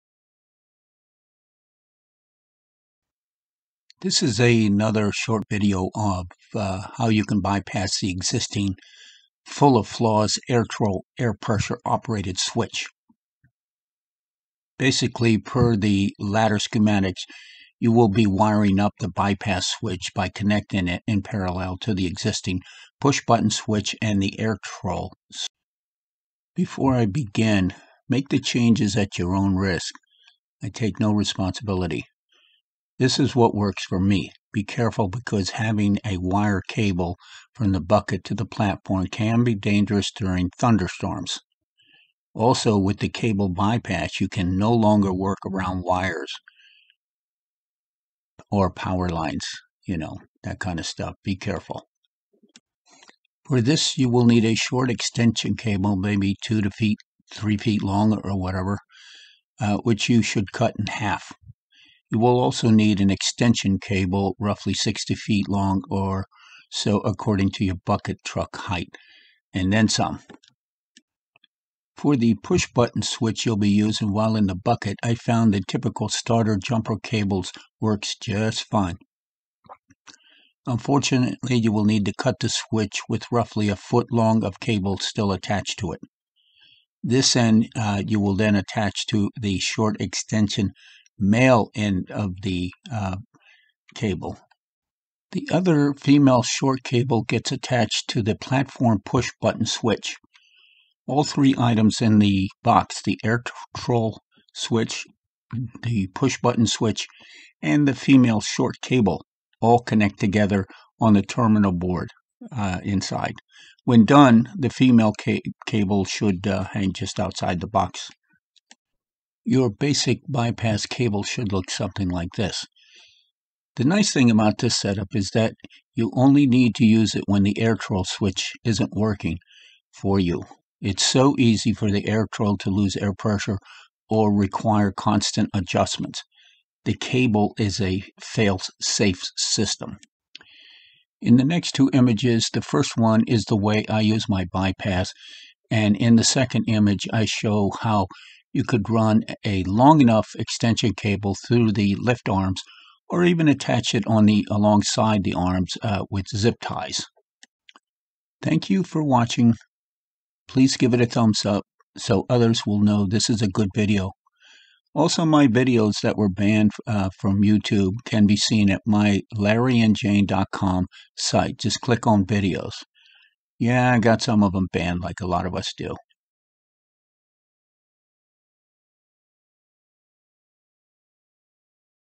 Bypass your Bucket Truck Air Pressure Airtrol Switch.
This is another remake of my original video, but this time it is a bit easier to understand. Cover all lifts that use air pressure to activate the platform controls.